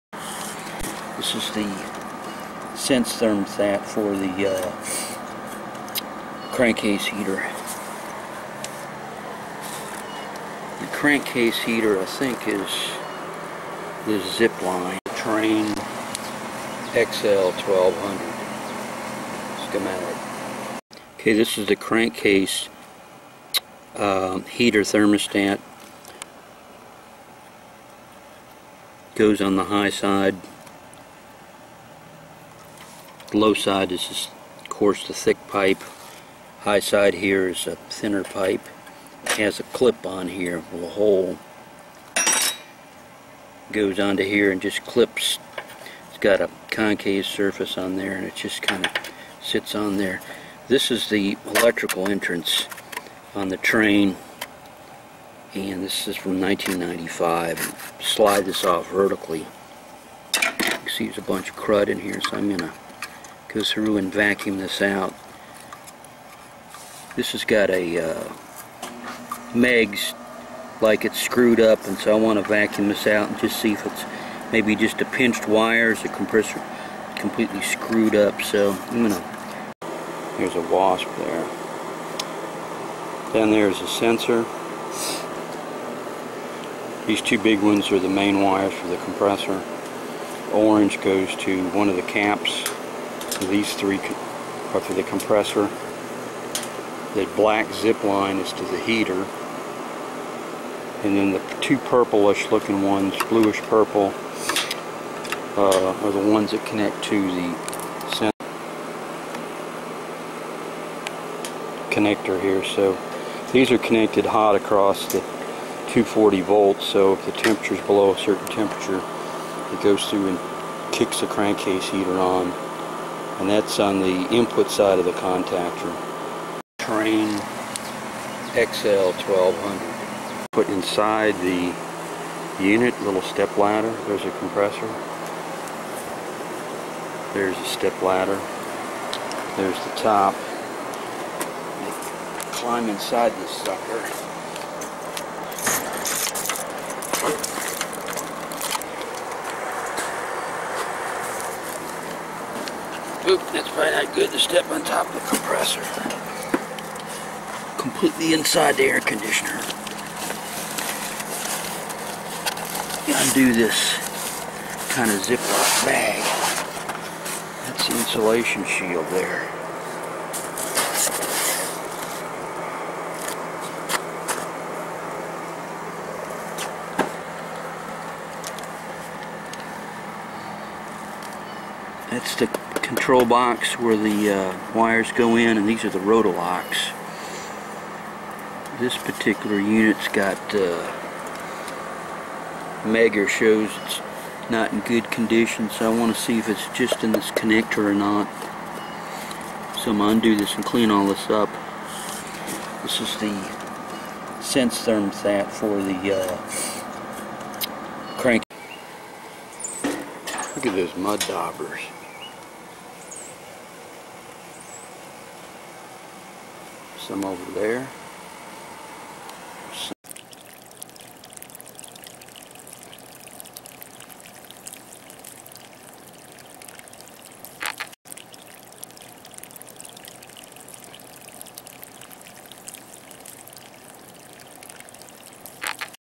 Trane XL 1200 Discharge Line Thermostat for Crankcase heater
THT00811 Trane Thermostat opens at 105F and closes at 73F . It connects to the Heater at the bottom of the Compressor. On this unit the the heater and thermostat are in series and are always connected to 240 volts since on the input side of the contactor. The sump heater HTR1298 is 65 watts at 240 volts.

This type of discharge line thermostat (DLT) is used to keep the compressor warm. Another type of DLT is used turn off the compressor is the Line is say above 260F (125C) . ie protects the compressor from excessively high discharge temperatures.

This video shows a Trane COM03694 compressor that is 5.3 tons using R22.